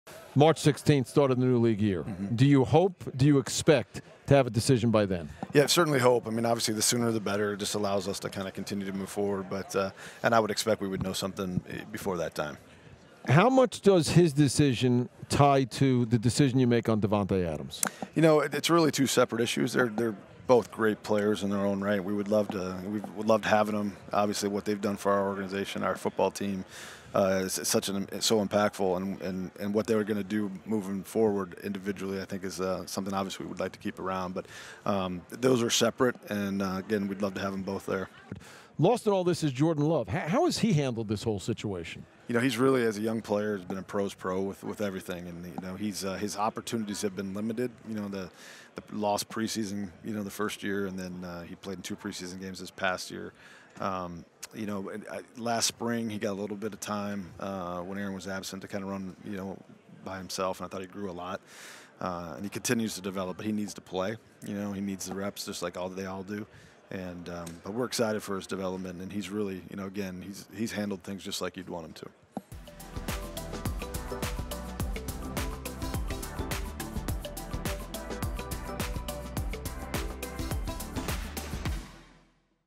Brian Gutekunst on Aaron Rodgers, Davante Adams, & Jordan Love | SiriusXM
Green Bay Packers GM Brian Gutekunst talks Aaron Rodgers, Davante Adams, and Jordan Love.

Hear more from SiriusXM NFL Radio on our app!